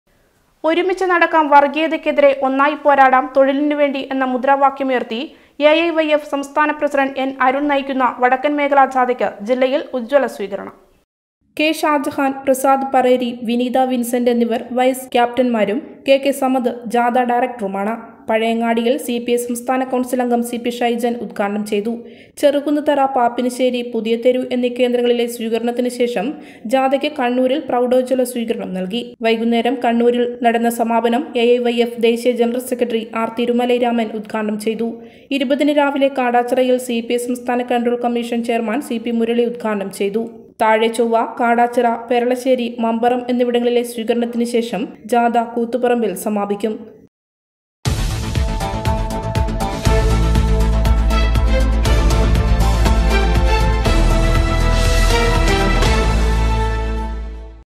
വടക്കന്‍ മേഖല ജാഥക്ക് ജില്ലയില്‍ ഉജ്ജ്വല സ്വീകരണം
ഒരുമിച്ച് നടക്കാം വര്‍ഗ്ഗീയതയ്ക്കെതിരെ ഒന്നായി പോരാടാം തൊഴിലിനു വേണ്ടി എന്ന മുദ്രാവാക്യം ഉയര്‍ത്തി എഐവൈഎഫ് സംസ്ഥാന പ്രസിഡന്റ് എന്‍ അരുണ്‍ നയിക്കുന്ന വടക്കന്‍ മേഖല ജാഥക്ക് ജില്ലയില്‍ ഉജ്ജ്വല സ്വീകരണം